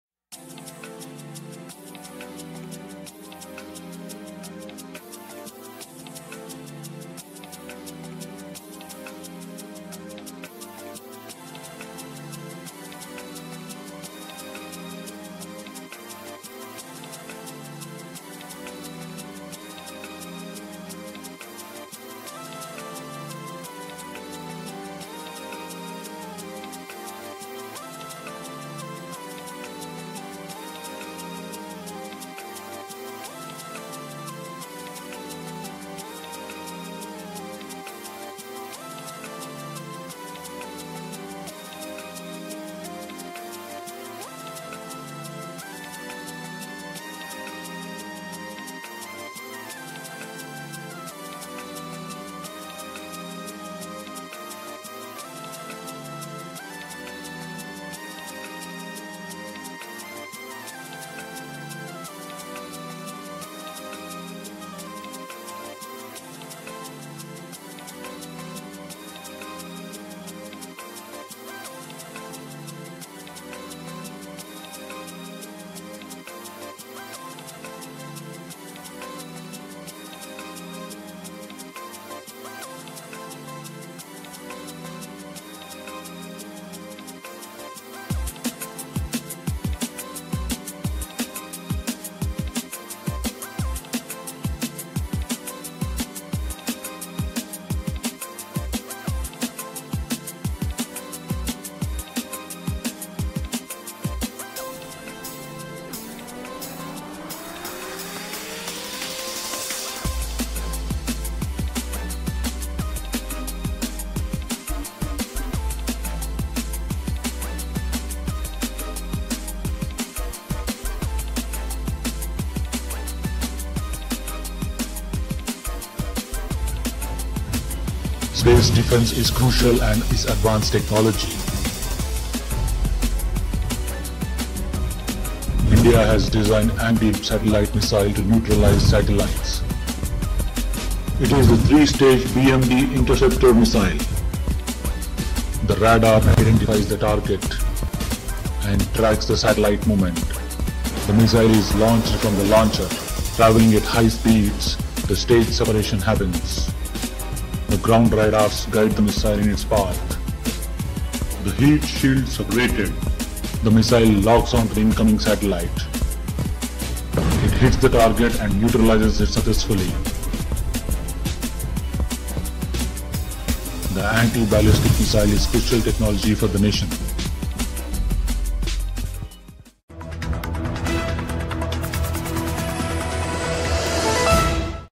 All your questions on Mission Shakti answered
On March 27, 2019 India conducted Mission Shakti  An Anti-satellite missile was launched from APJ Abdul Kalam Island by DRDO